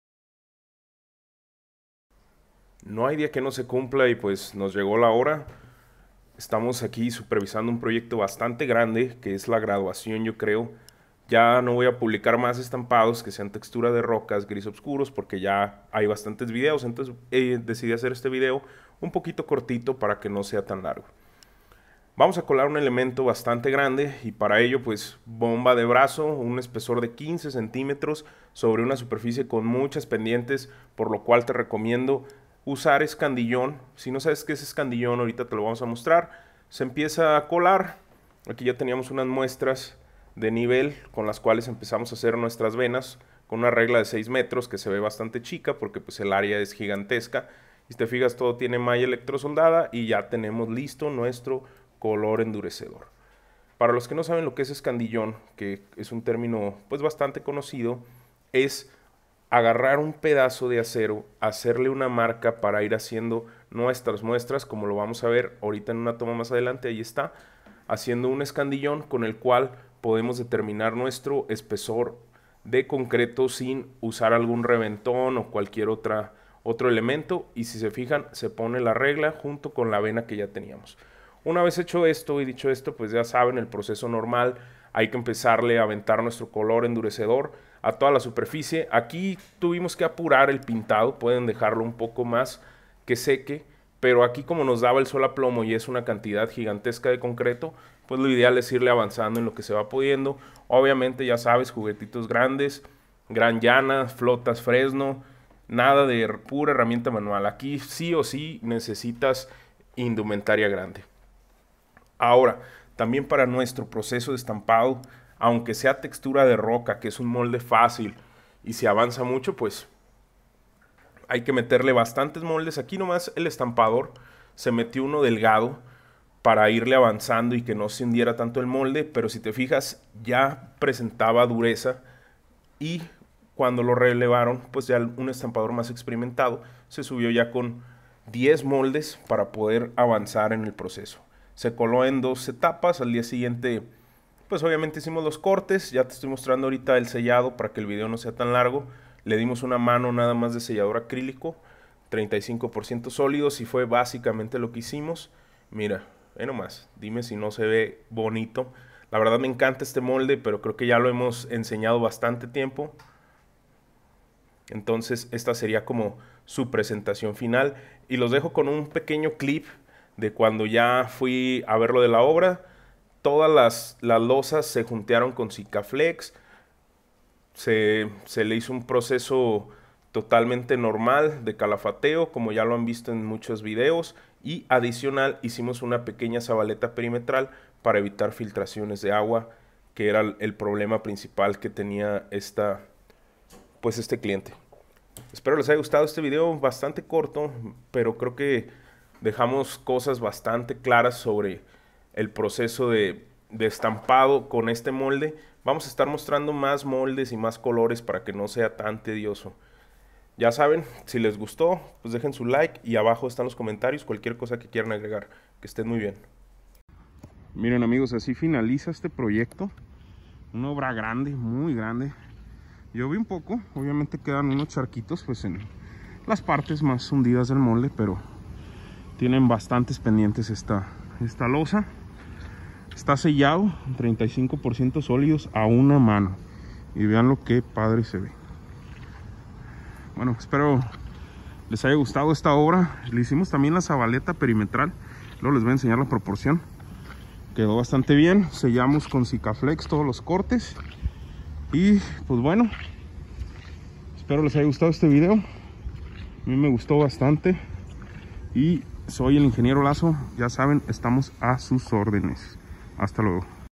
CONCRETO ESTAMPADO TEXTURA DE ROCA O PIEL DE ELEFANTE COLOR GRIS OBSCURO.
Que tal amigos espero les guste este video donde hacemos practicamente la graduacion de este color y molde y vamos a comenzar a compartir mas moldes, colores y tecnicas.
una disculpa me parece un video muy cortito, haganme saber en los comentarios que tipo de videos lesgustaria ver.